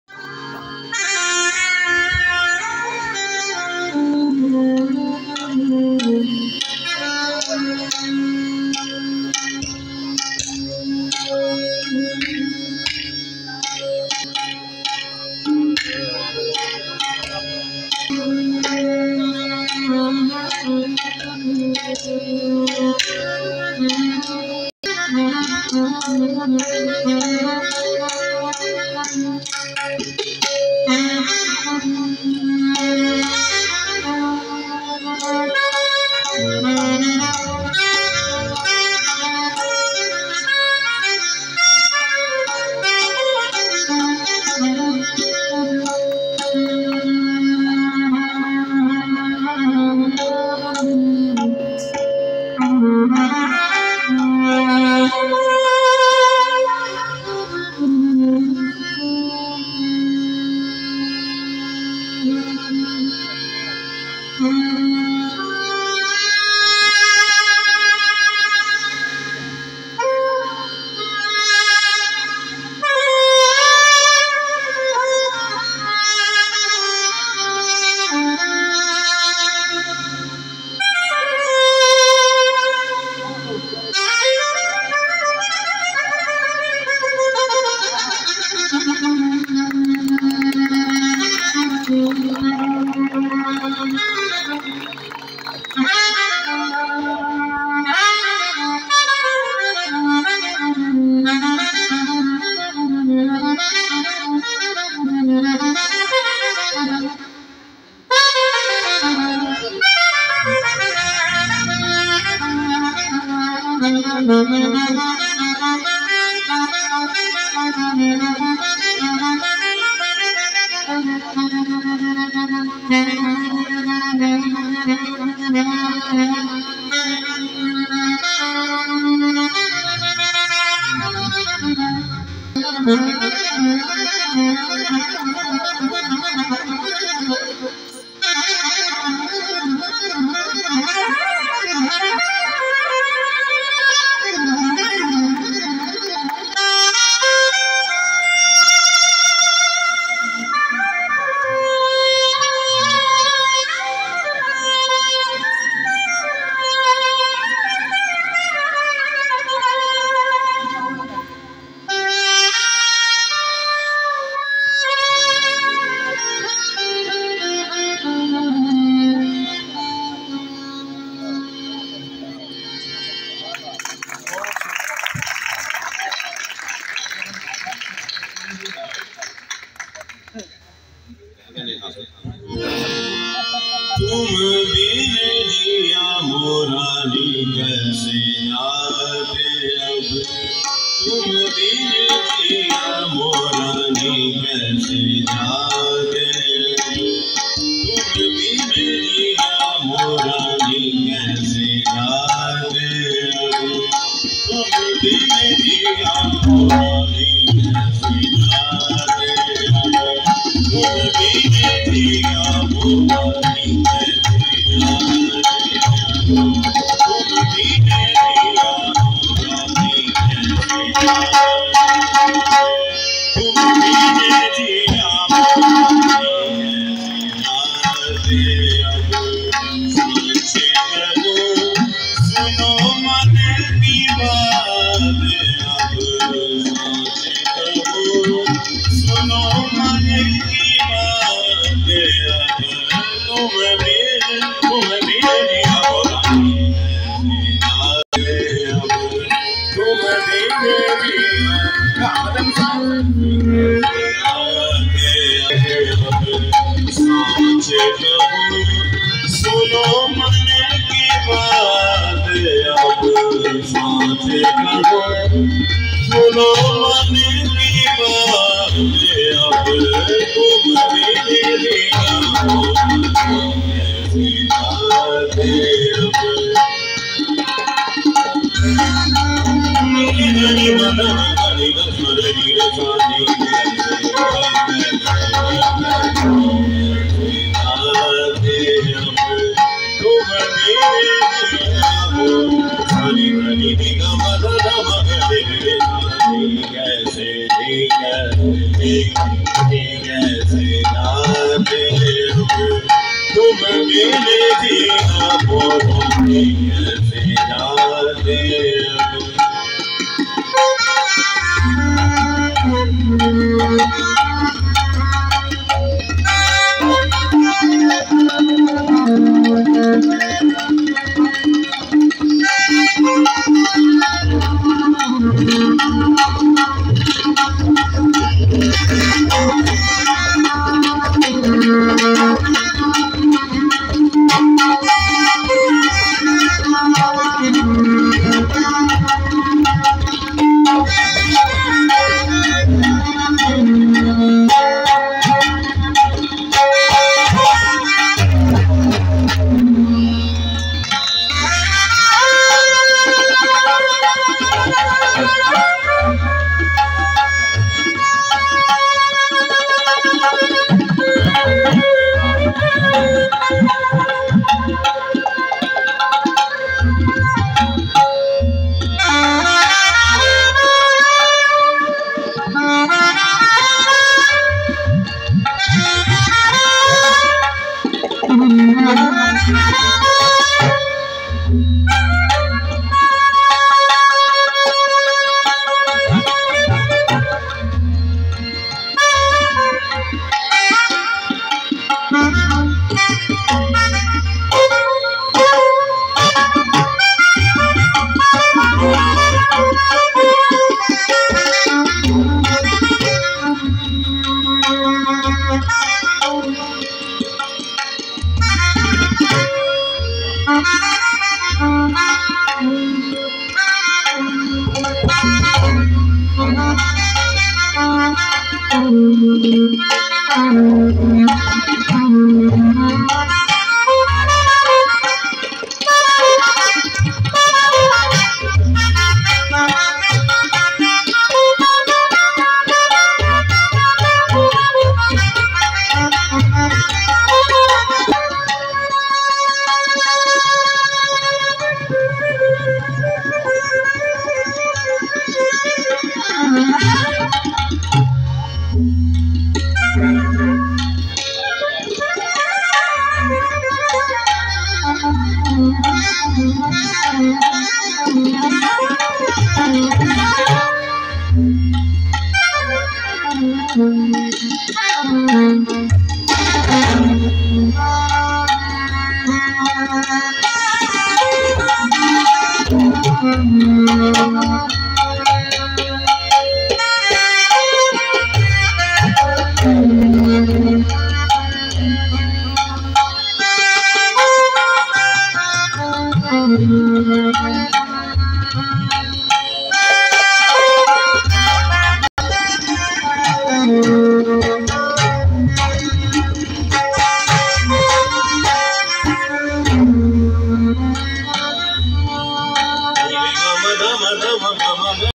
saxophone player Ustad AR chhemo khan and tabla player Sani Jimmy live classical music
AR khan G